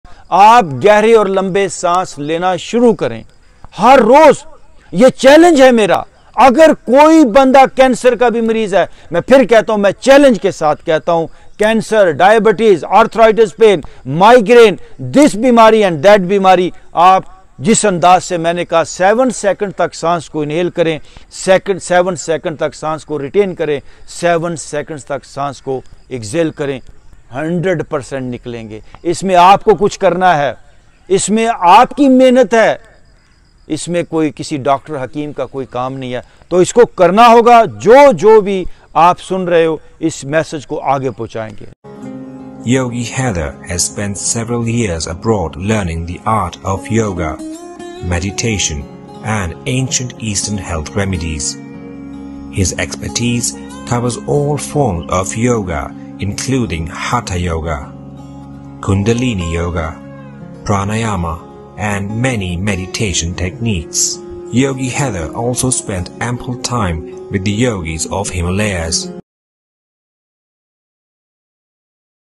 Healing Through Breath | Get Rid of All Diseases Without Medicine | Yogi Haider
Yoga Breathing
Pranayama
How to heal without medicine
Benefits of deep breathing
Yoga & Meditation is a way to achieve self Insight, Mind & Body Harmony & absolute cure for all Physical & Psychological Disorders.
We can improve our heart and lungs functioning with these simple exercises .